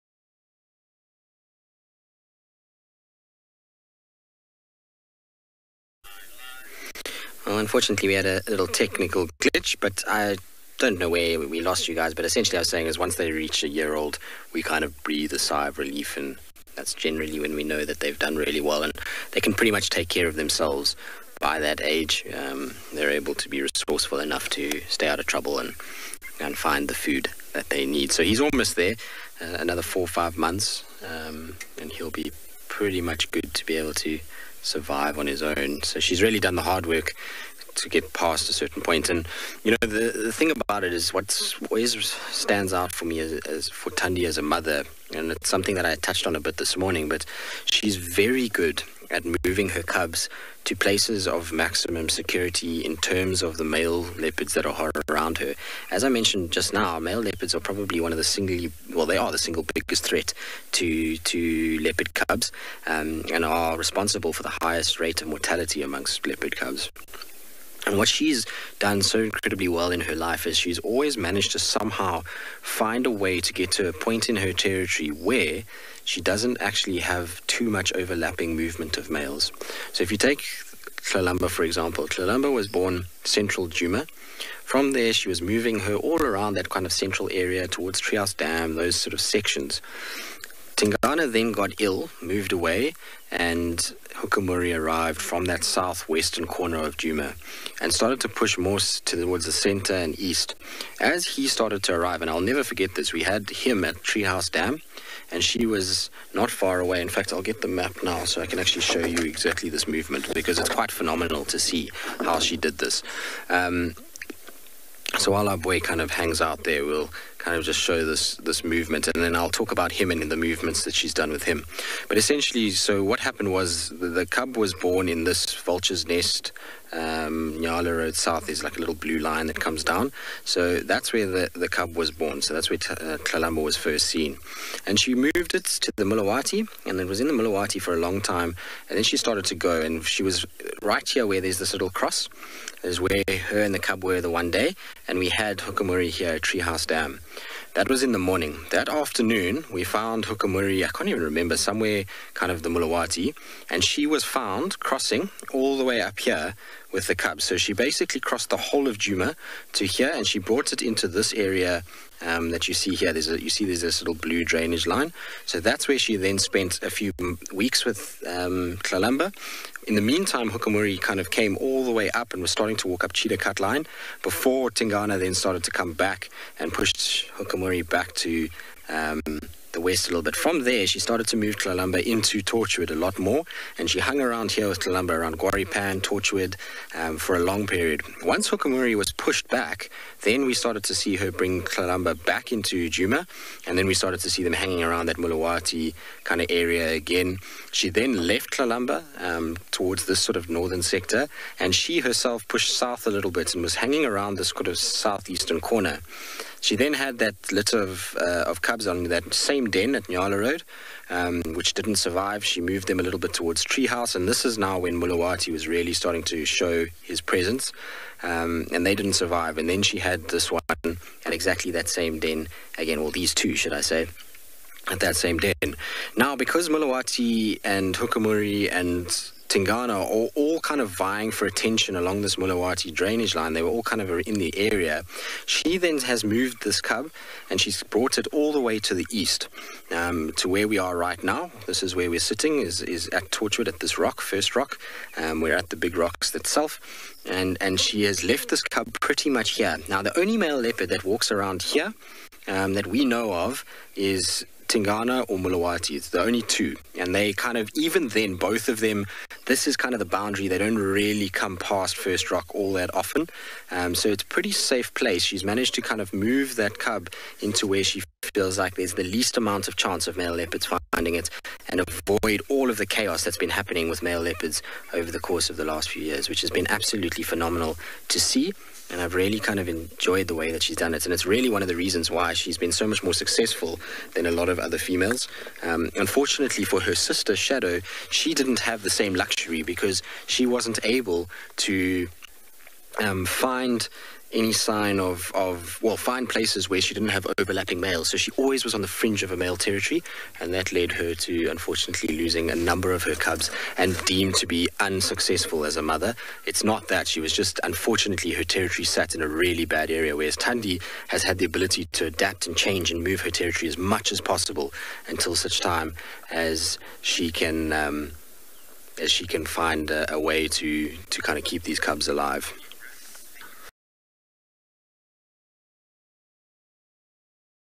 Highlights Tristan discusses Thandi's mothering technique 8 Dec 2020
Tristan is with Thandi's Son and discusses Thandi's mothering technique 8th December 2020 Djuma private game reserve south africa.